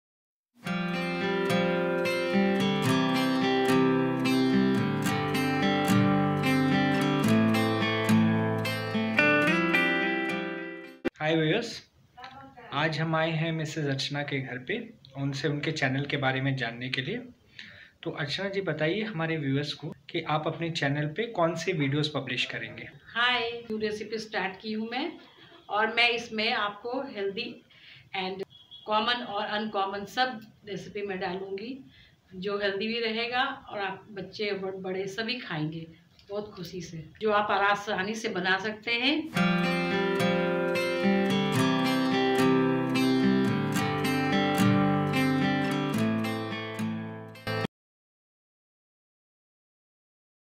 Channel Trailer - Archana's Healthy Recipes
Hi! I will post regular videos on healthy recipes that are easy to cook at home, giving information about their health benefits. Do comment on my videos here on how did you feel after cooking a recipe, & share the video with others.
Happy Cooking!